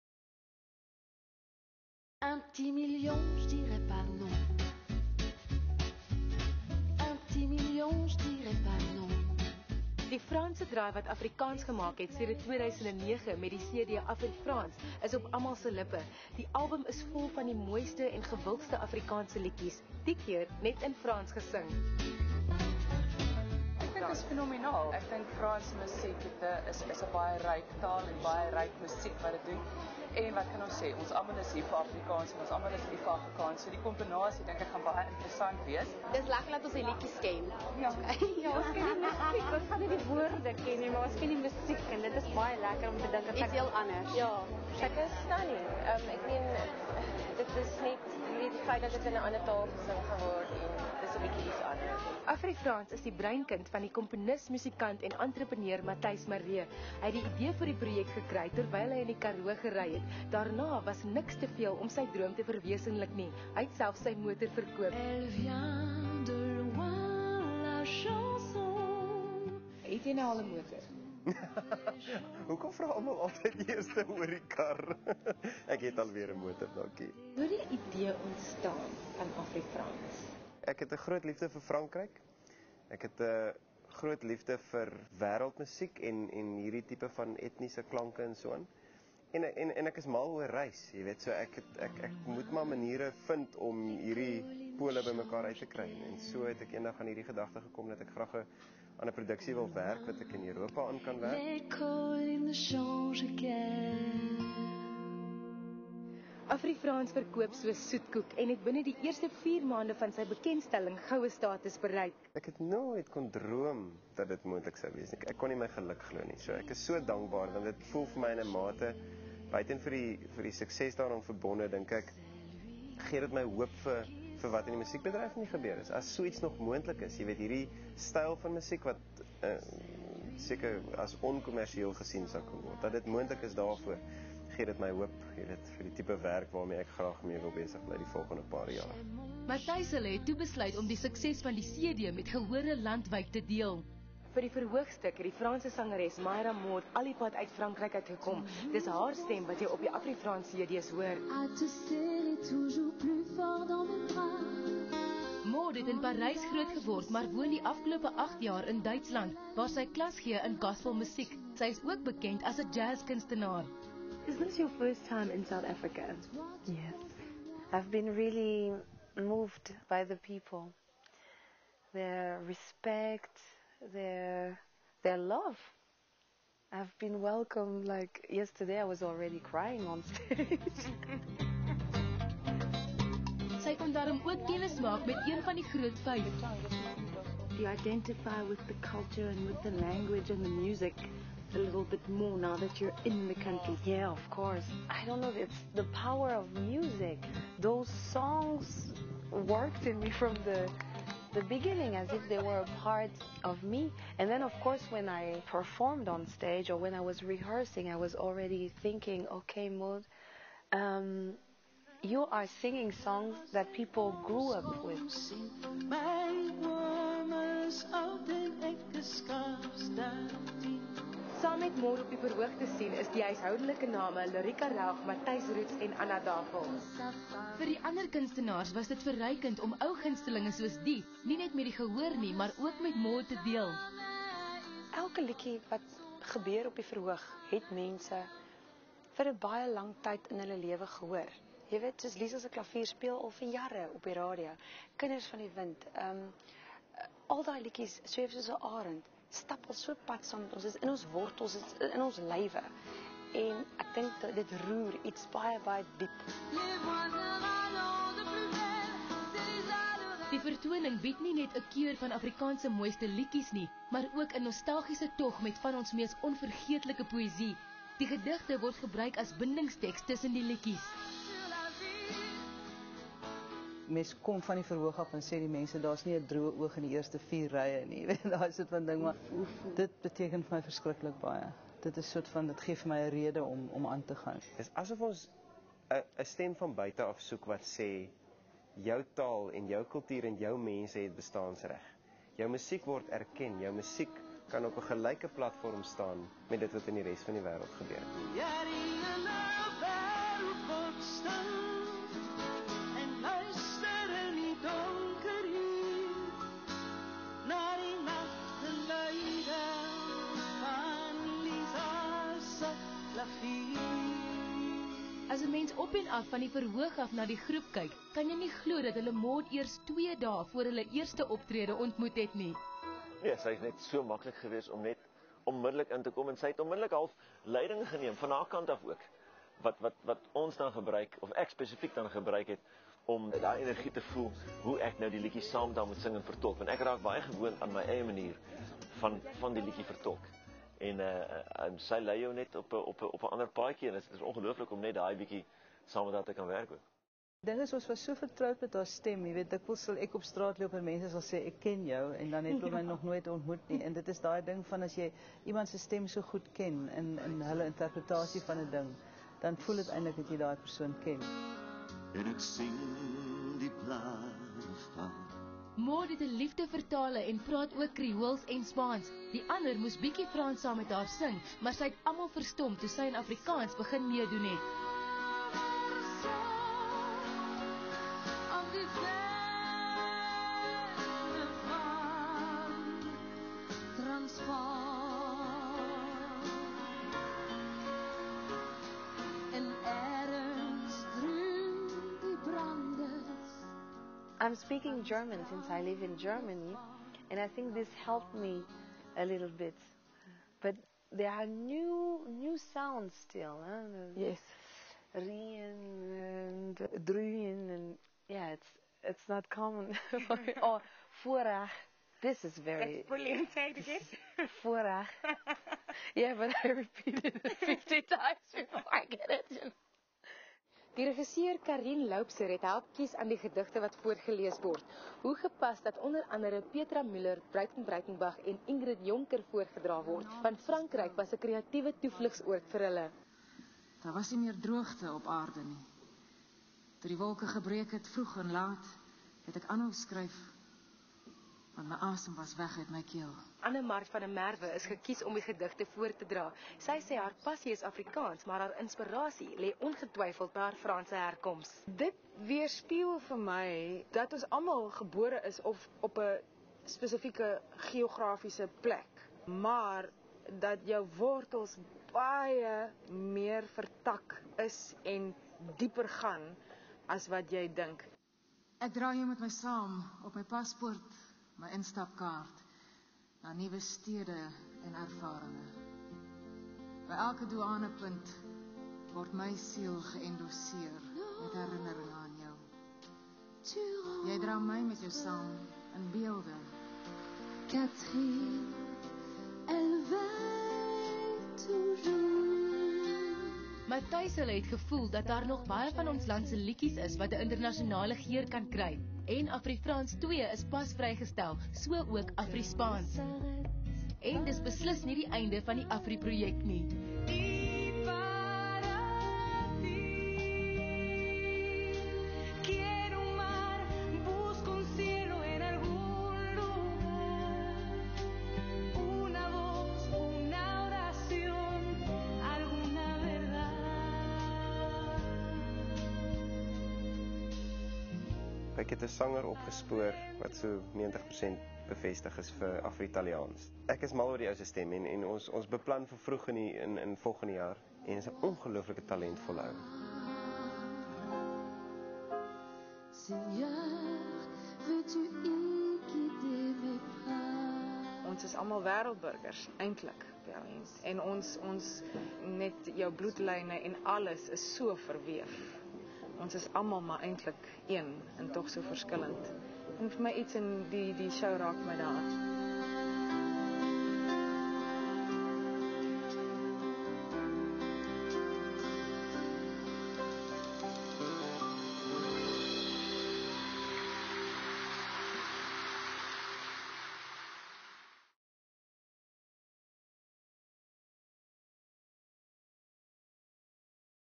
Afri-Frans "The Kwela Insert"
Interview of the different performers of the Afri-Frans Show at the KKNK Festival  in Oudtshoorn (South Africa) in April 2010